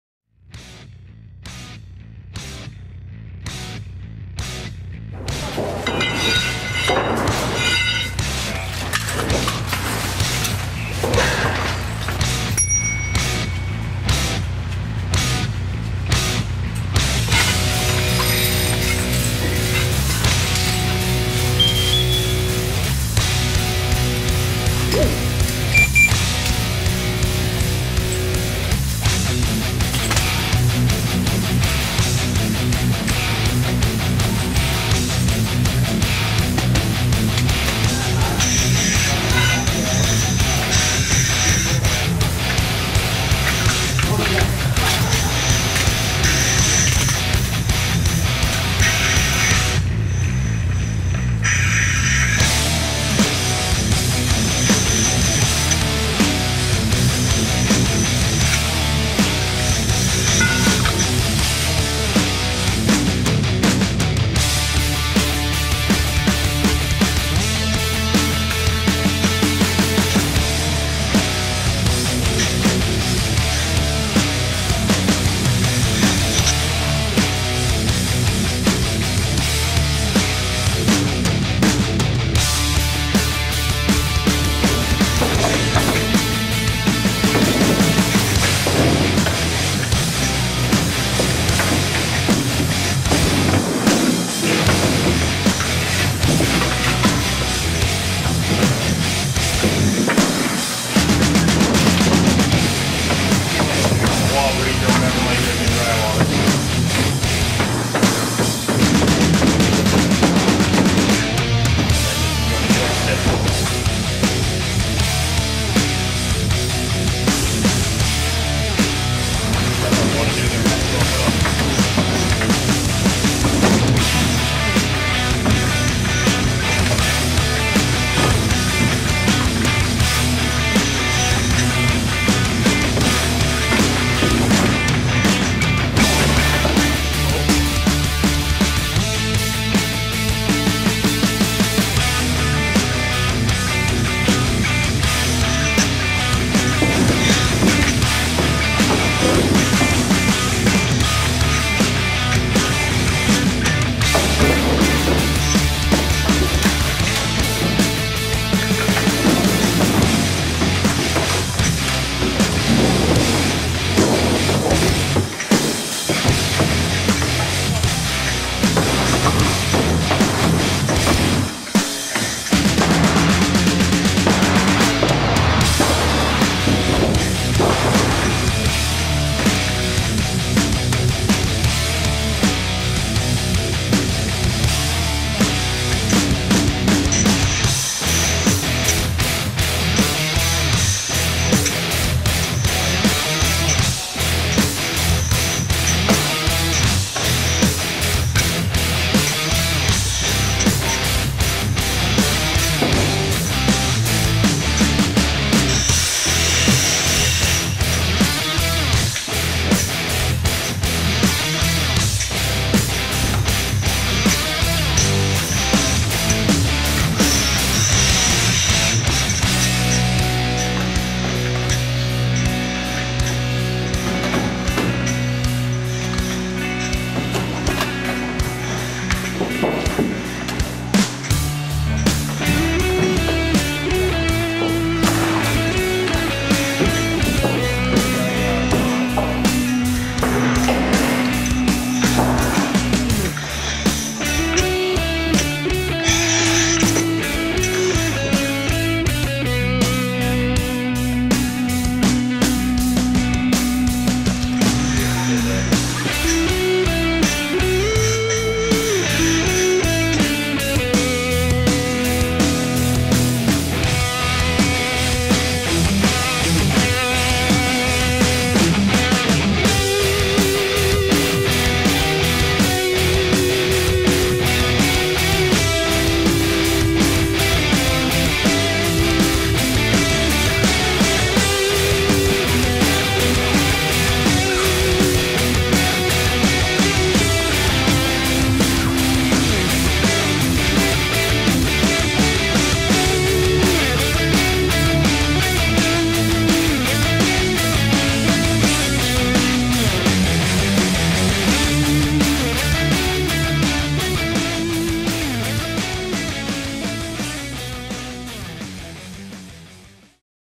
SCBA and Radio Competency Drill
SCBA and radio competence drill performed on the self rescue training prop.